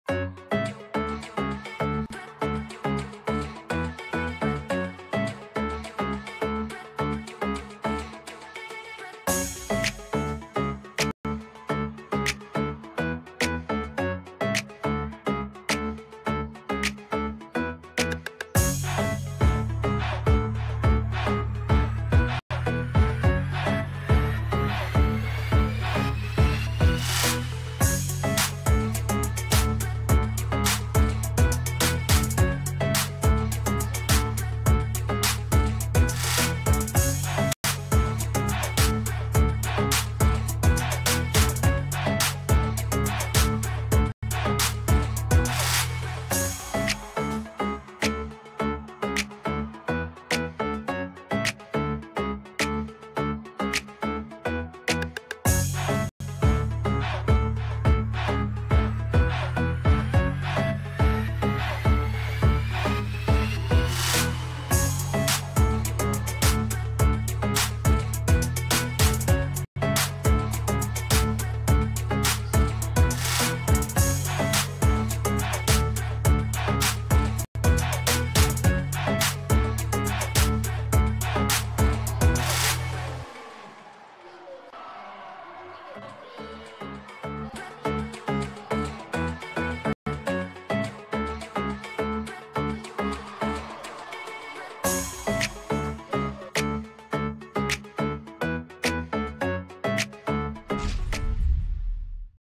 Arapahoe vs. Denver East - Girls Varsity Basketball HUDL Highlights | 2/21
Highlights created by HUDL.